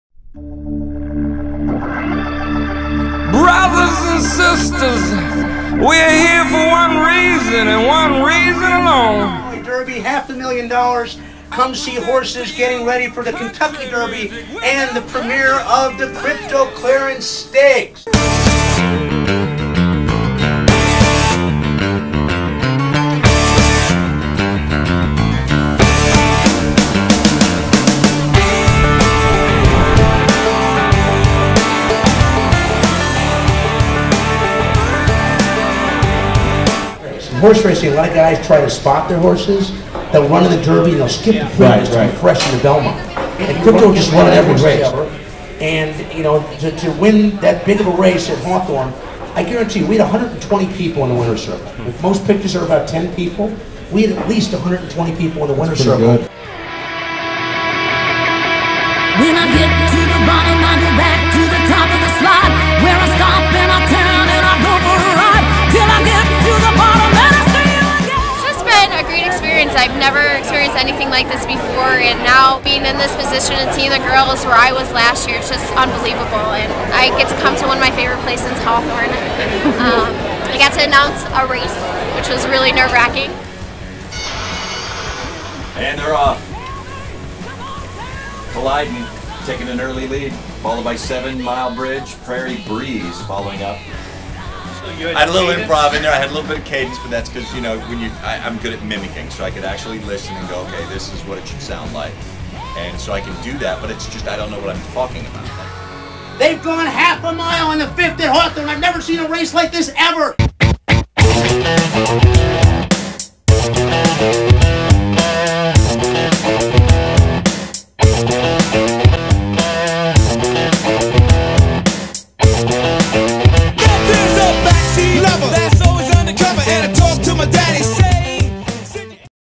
Illinois Derby 2010
Relive the 2010 Illinois Derby at Hawthorne Race Course with those who starred in it, including ESPN1000 host Harry Tenowitz, 97.9FM The Loop host Eddie Webb, Leah, The Loop Rock Girl 2009, 32 Loop Rock Girl contenders, thousands of fans and the winning horse, American Lion.